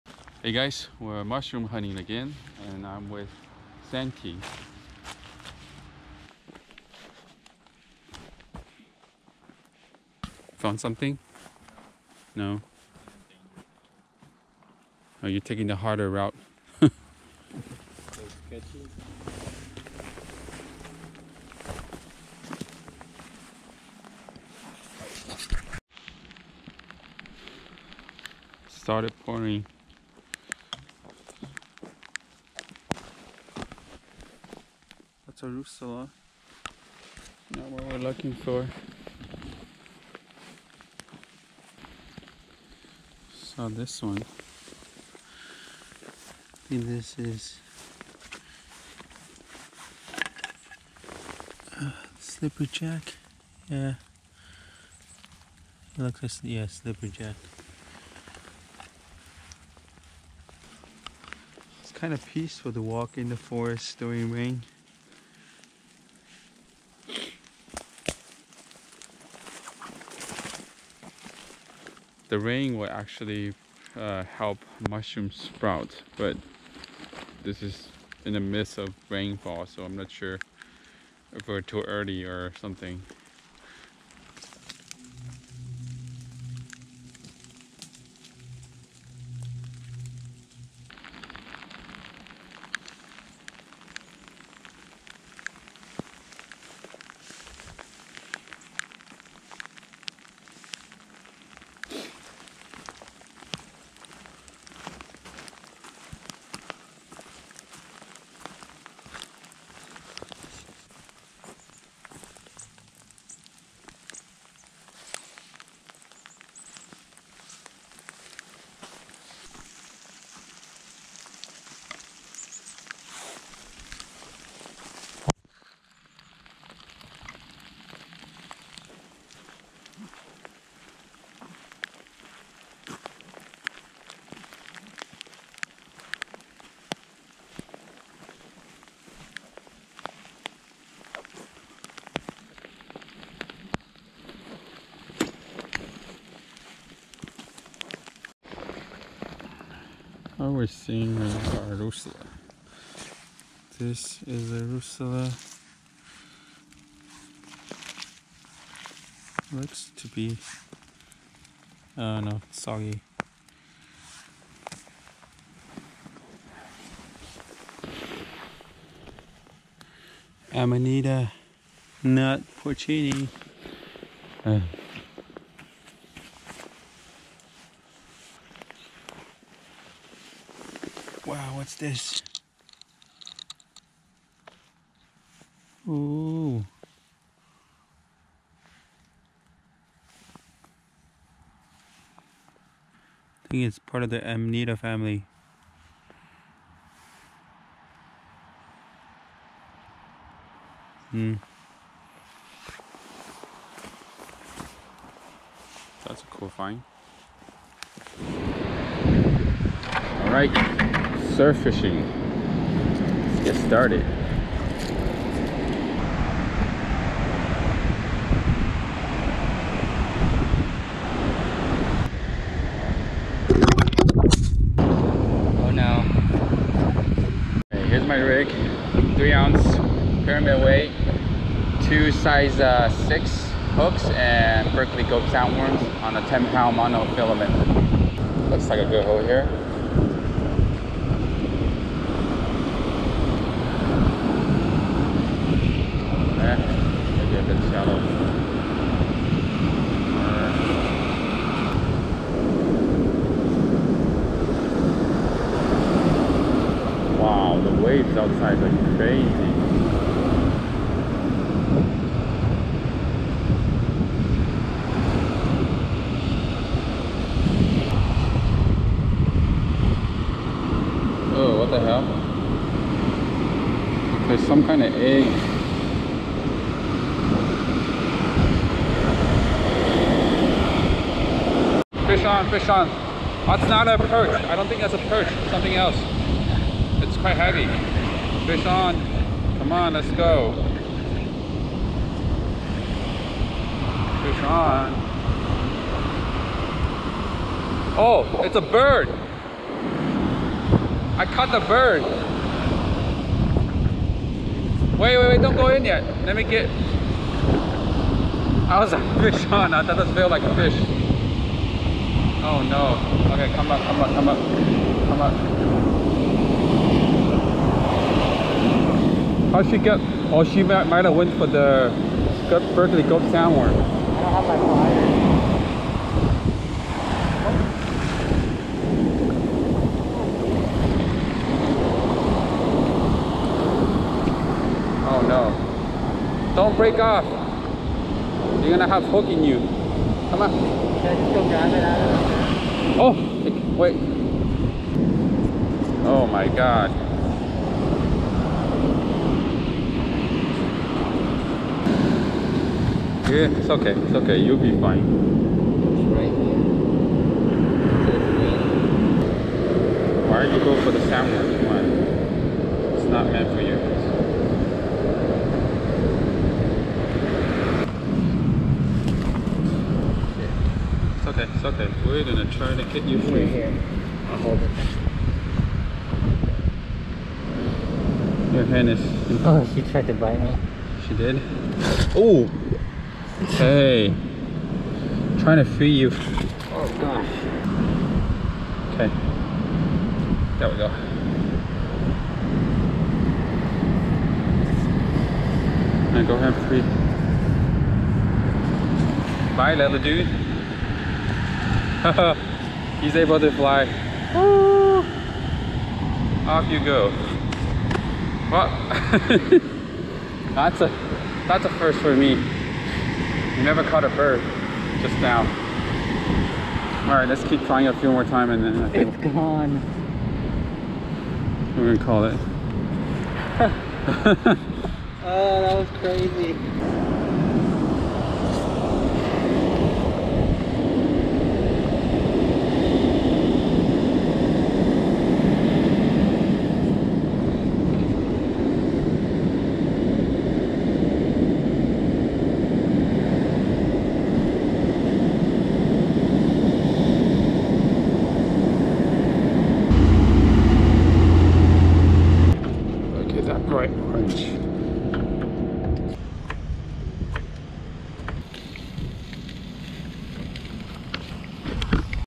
Raining Mushroom Foraging Plus A Surprise Encounter On the Surf
this was film back in Feb-March of 2021.
Only got around to edit it now.

enjoy haha
Thanks Santi for the invite!